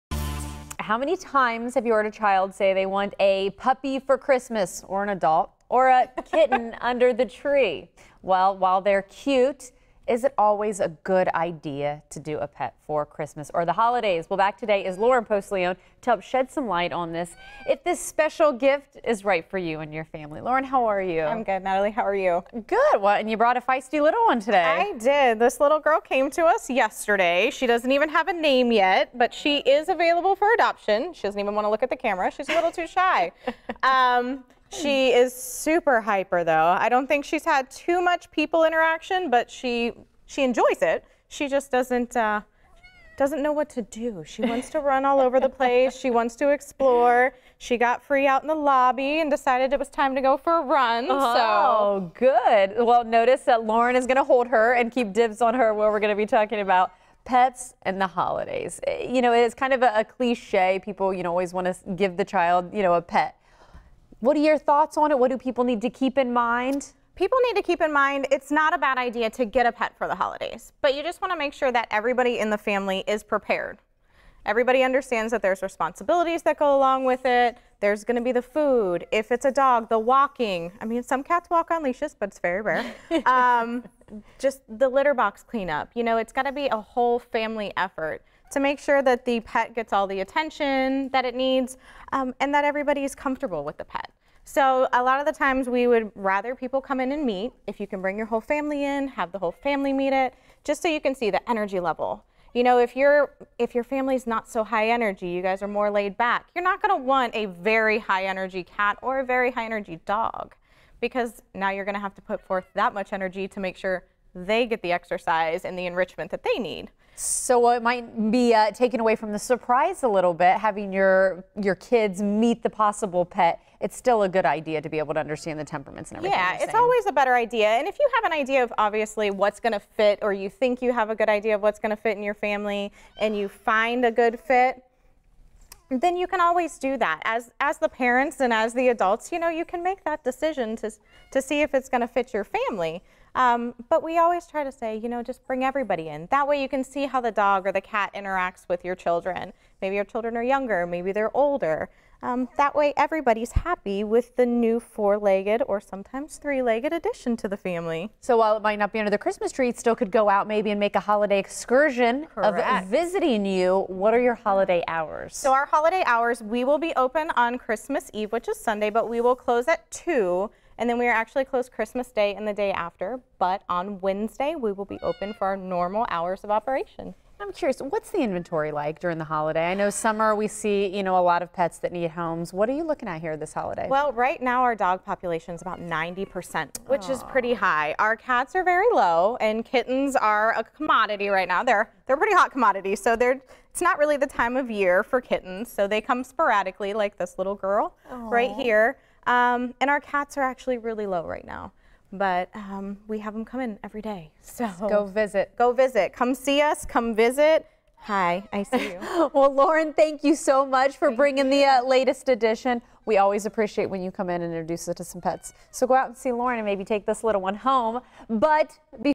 Pet Patrol
We try to find a forever home for this little kitten.